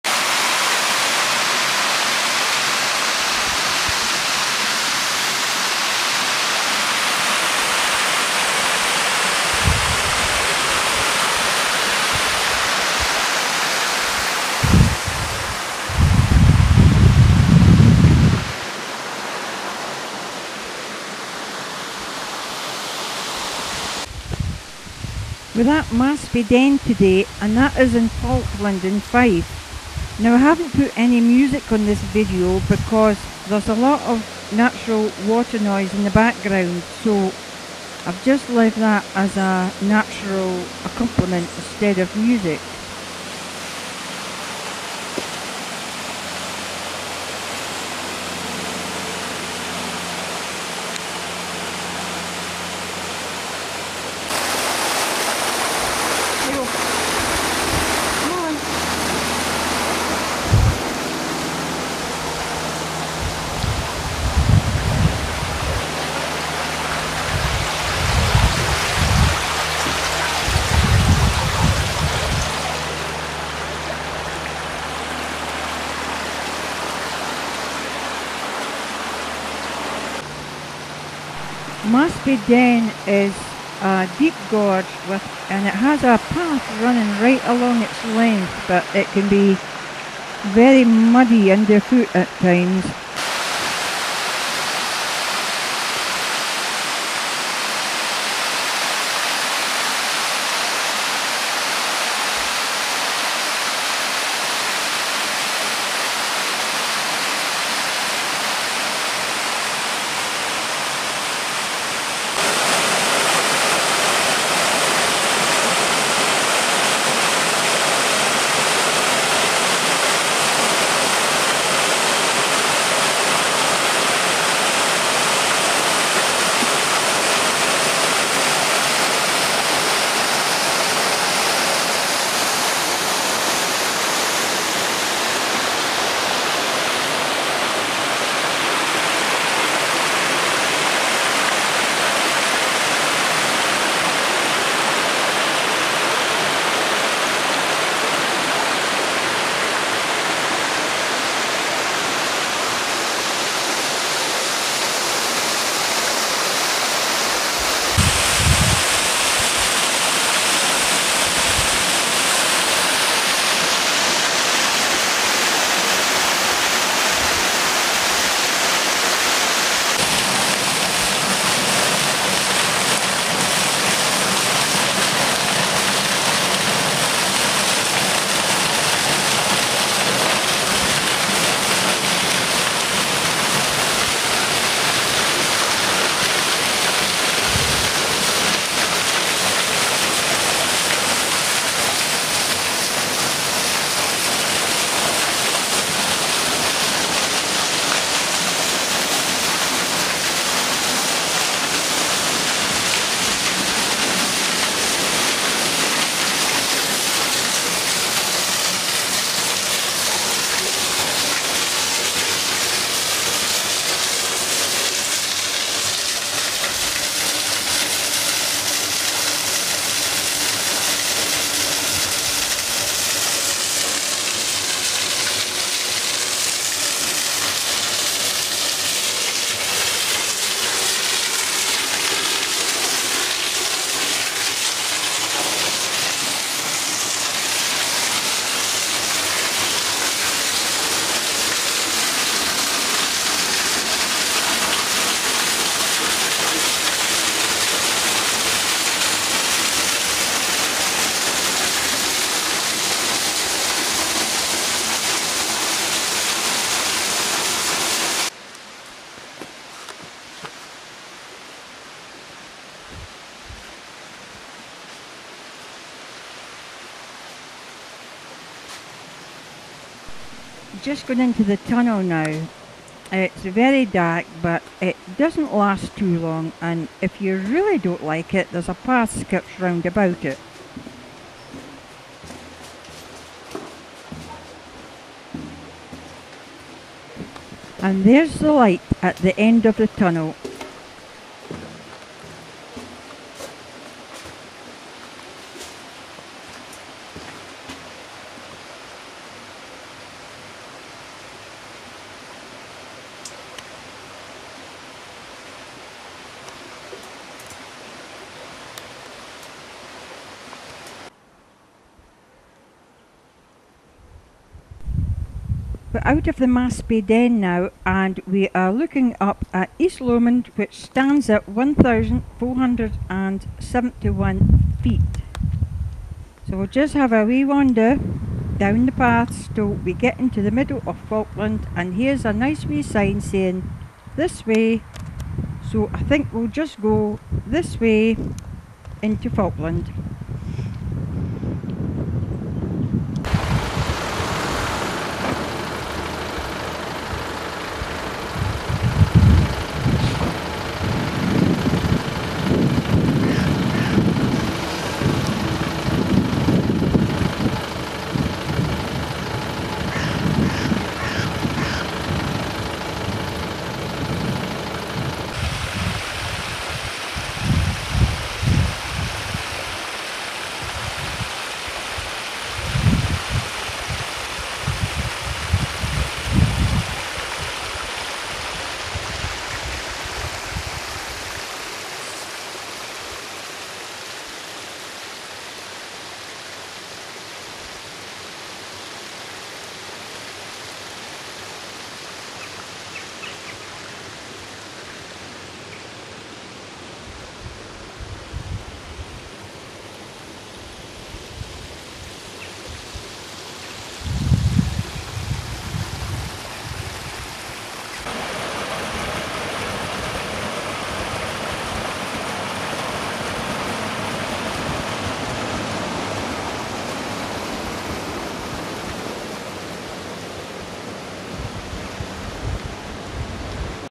Maspie Den, Falkland, Fife.
Maspie Den takes you across victorian footbridges made of stone and through a tunnel carved in the rock.  Falkland was the first conservation village in Scotland.  When you enter the village it is almost as if you have been transported back through the pages of history to medievel times.  There are cobbled streets and old buildings, some of which have
towers, popular in the medieval age.  Falkland has been a royal place since the days of the Stewarts.  King James 1V completed the main structure of the palace and King James V also added to the building there, transforming it into a sophisticated Renaissance palace.